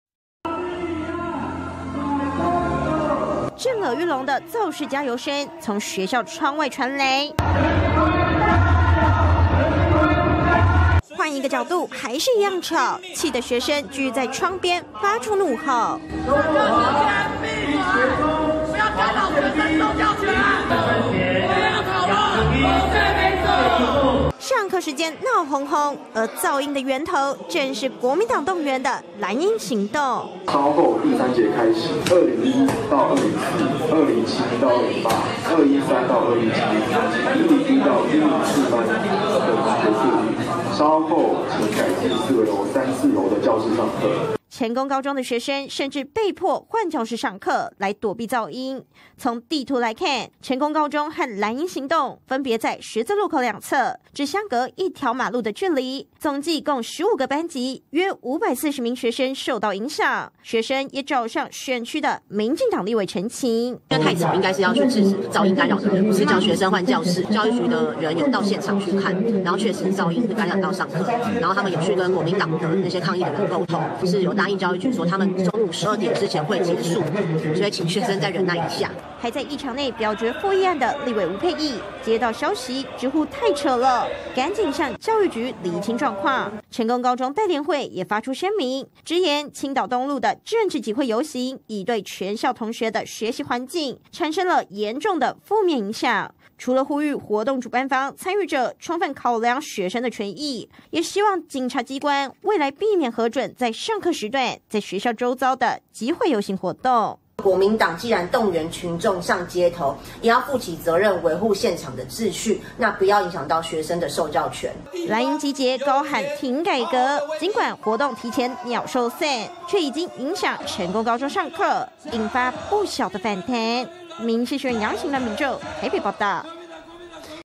國民黨動員藍鷹對決青鳥 造勢宣講干擾學生上課－民視新聞
【民視即時新聞】政治中心／楊喻晴、欒秉宙 &nbsp; &nbsp;台北報導今天國民黨動員基層「藍鷹」搭遊覽車到立法院外，但青島東路主舞台高聲造勢、宣講，卻吵到對街的成功高中學生，不只15個班級、約540名學生被迫換教室上課，他們也錄下噪音影片，向該選區立委吳沛憶陳情，成功代聯會更發聲明直言，該遊行已對學生產生嚴重的負面影響。